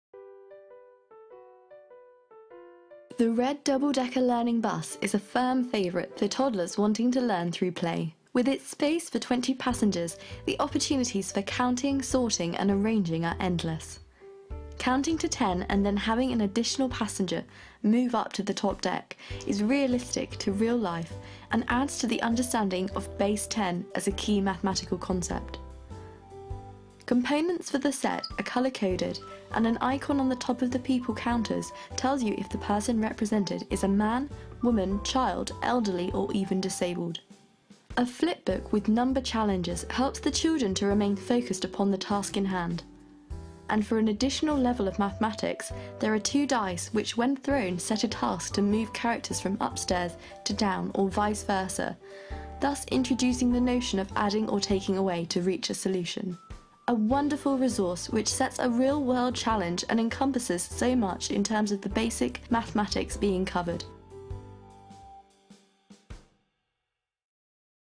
Drewniana gra edukacyjny autobus dodawanie odejmowanie Masterkidz 1-20
Cudowna gra edukacyjna w formie autobusu, który nauczy Twoje dzieci matematyki! Pozwoli nauczyć się działań na liczbach od 1 do 20. W grę wchodzi odejmowania i dodawanie.